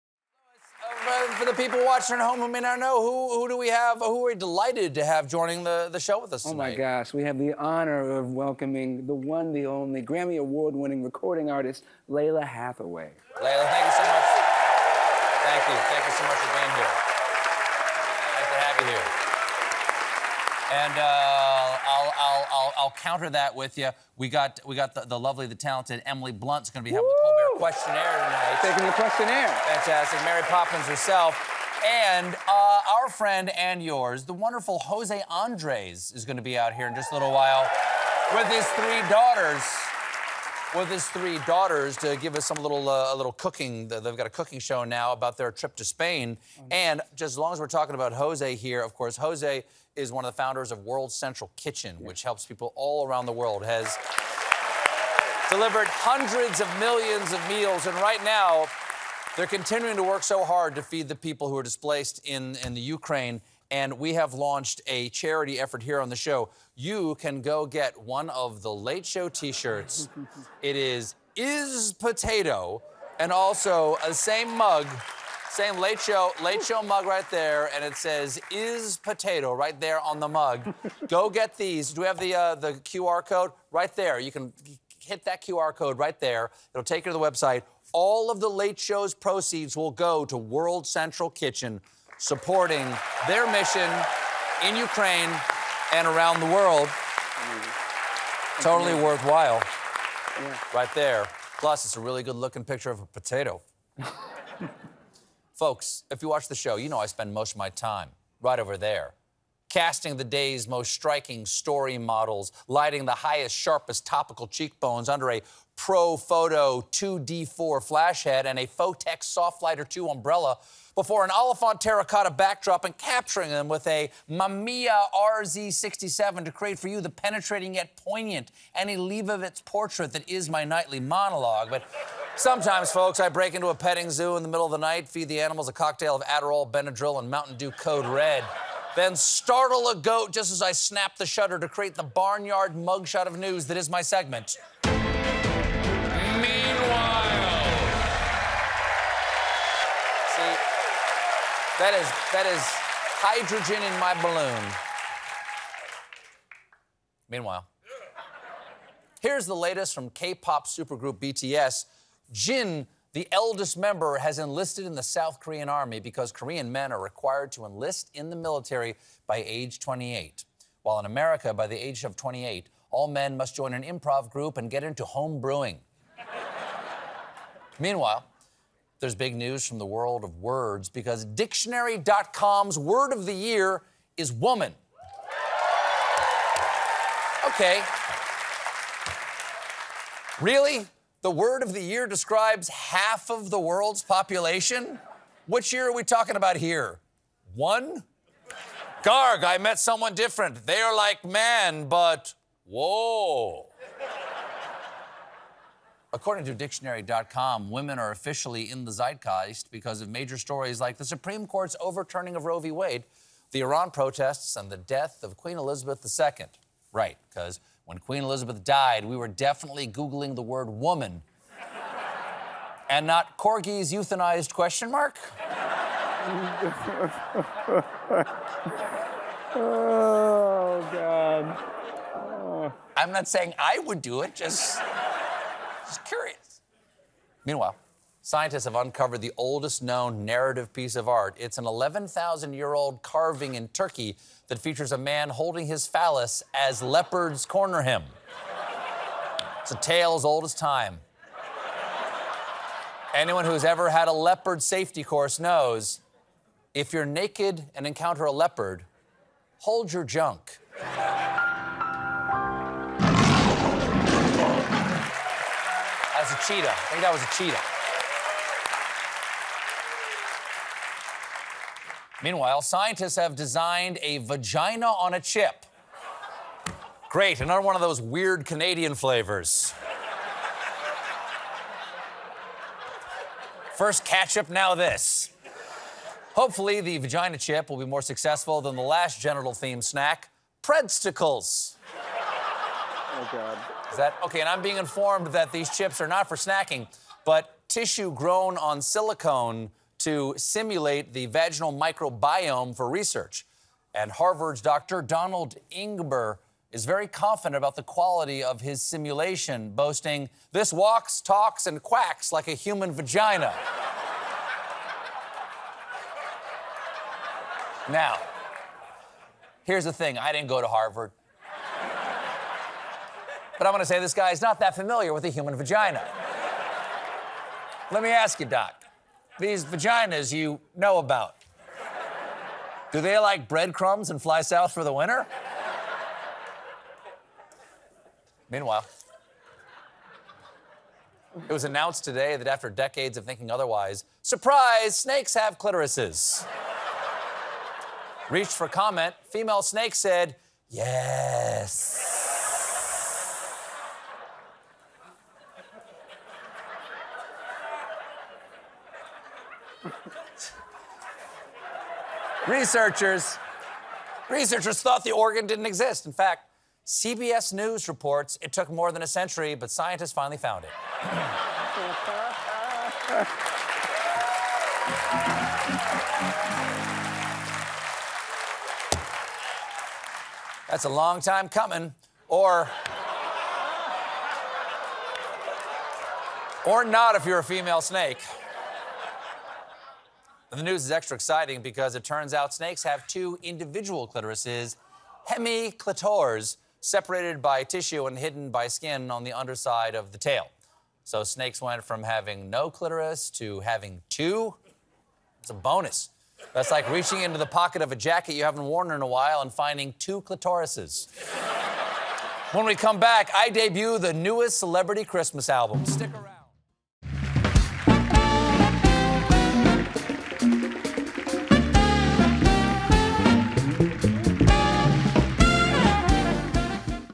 Meanwhile… BTS Star Jin Joins The Army | Vagina On A Chip
Meanwhile… K-pop superstar Jin from BTS has enlisted in his country's army, and scientists have designed something called "vagina on a chip."